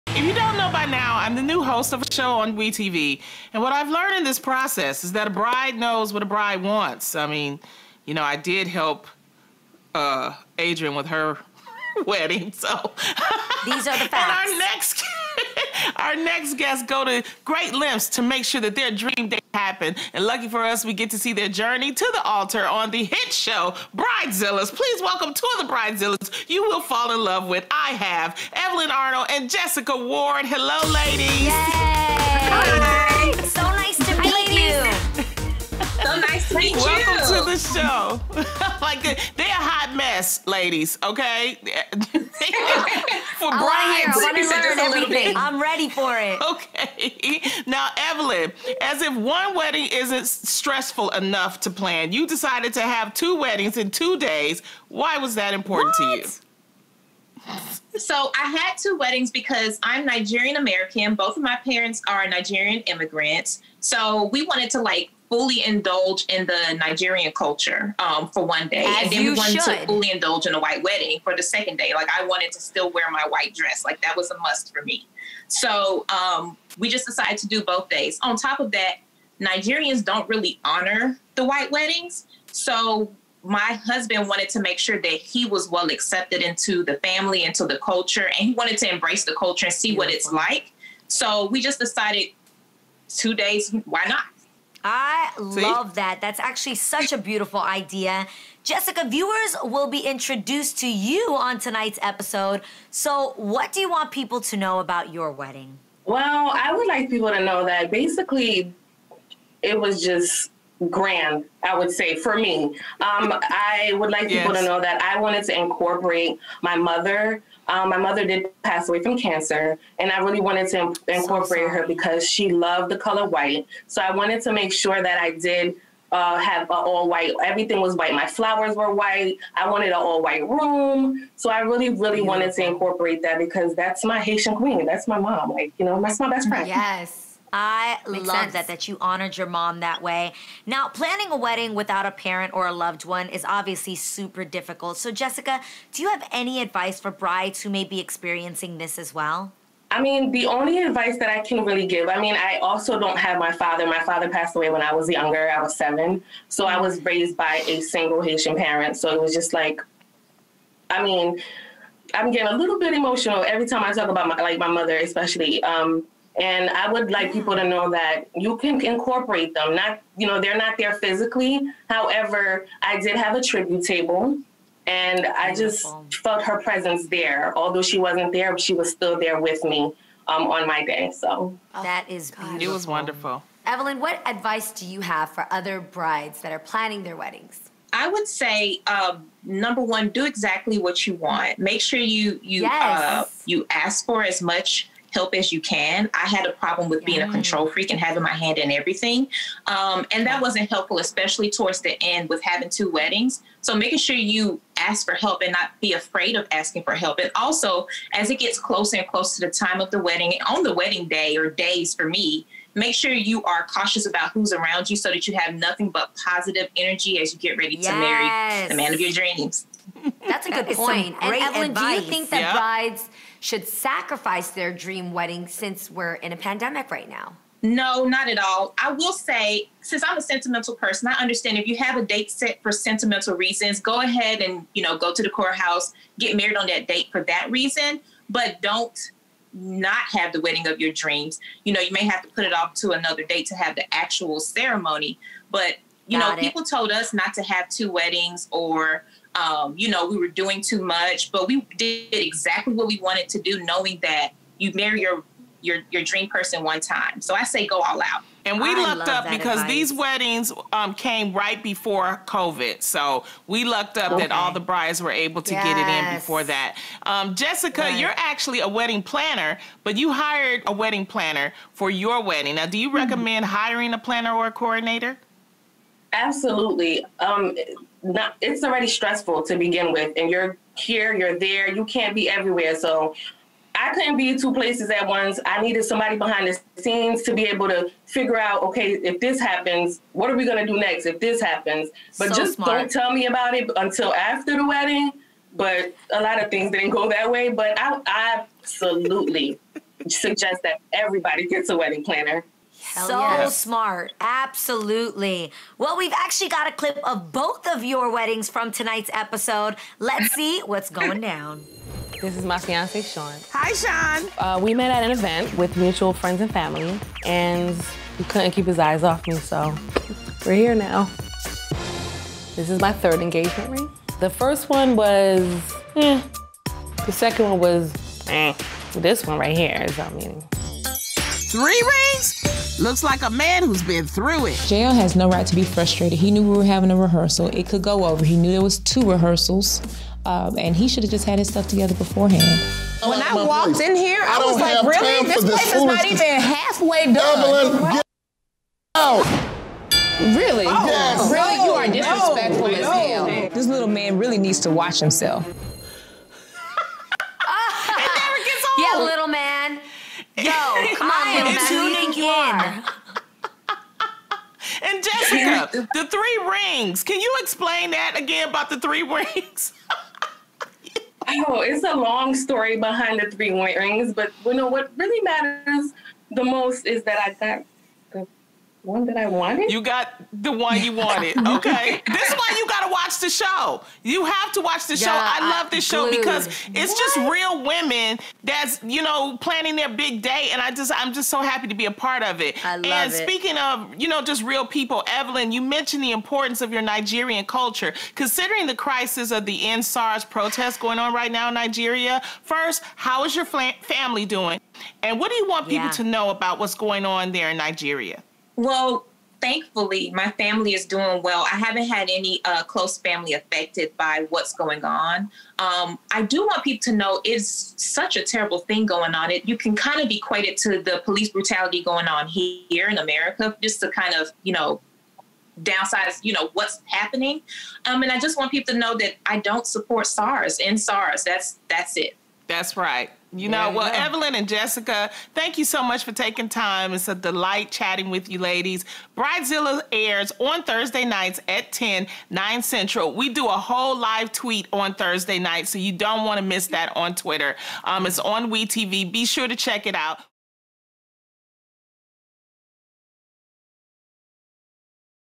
‘Bridezillas’ Stars Evelyn Arnold & Jessica Ward Stop By!
Evelyn Arnold and Jessica Ward share with us whether brides should sacrifice their dream wedding during the pandemic, plus so much more!